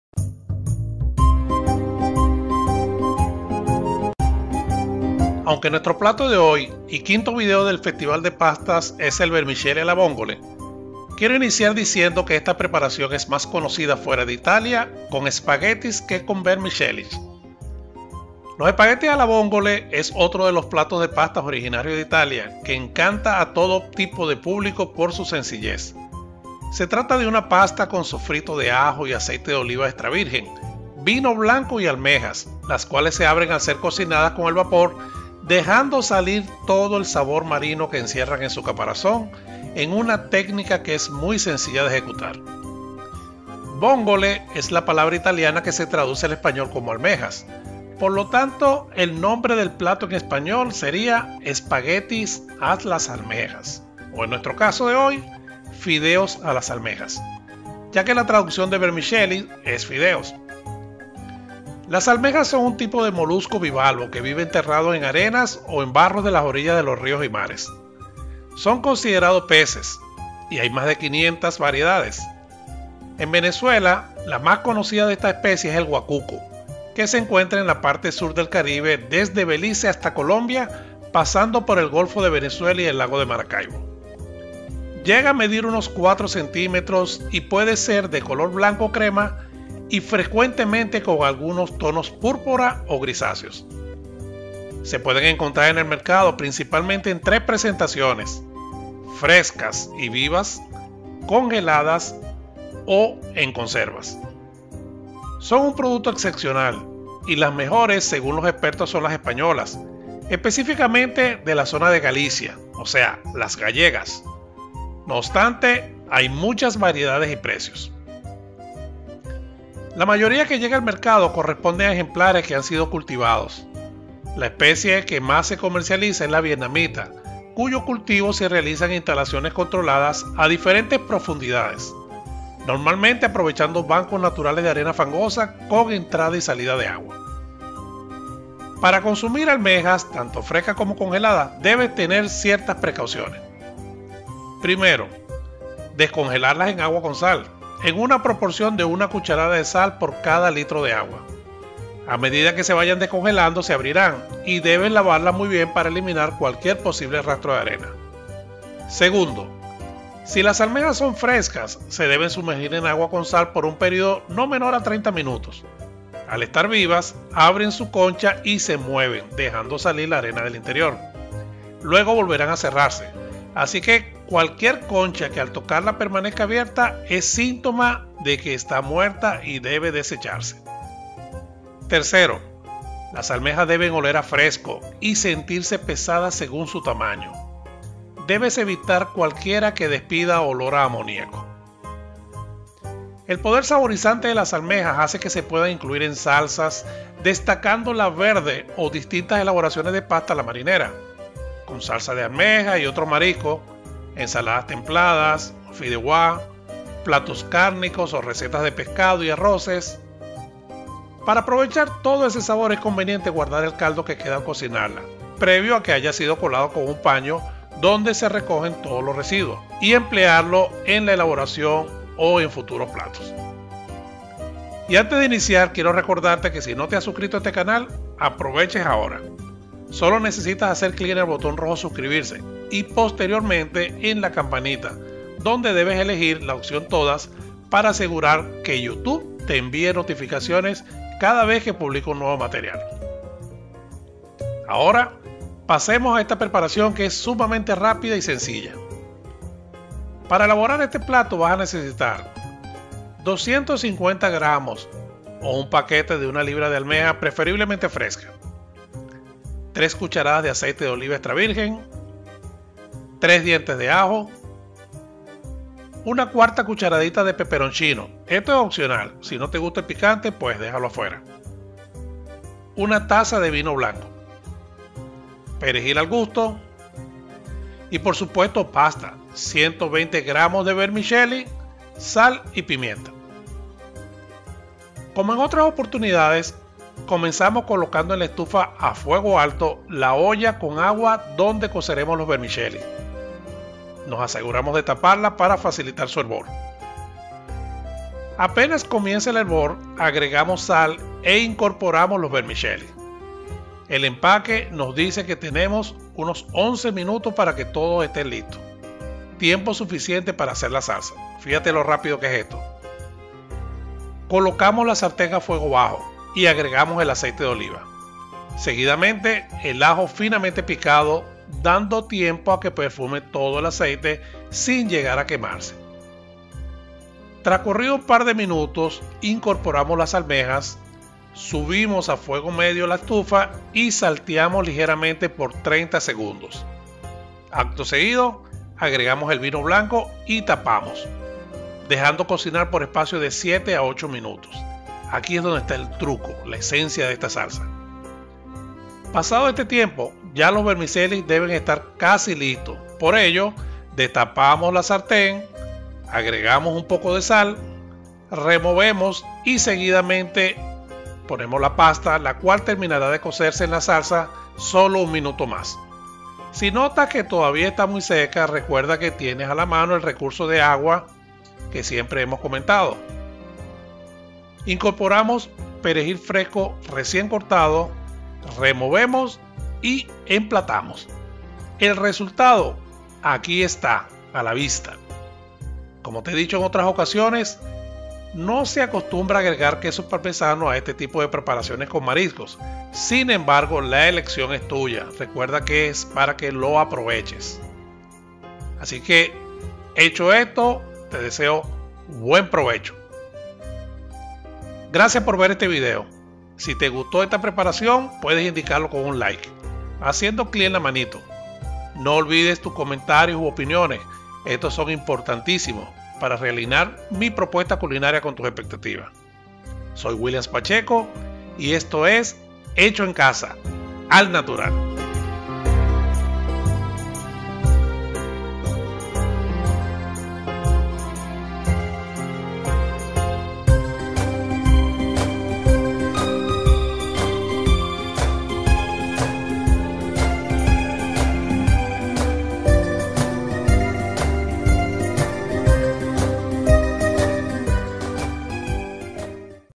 Vermicelli alle Vongole
En este video aprende a preparar de forma rápida y sencilla los Vermicelli alle Vongole, en español, fideos a las almejas. Este plato de pasta te entrega toda la esencia del mar en un bocado

Conoce la historia de como surgió. Es es el 5to video del Festival de Pastas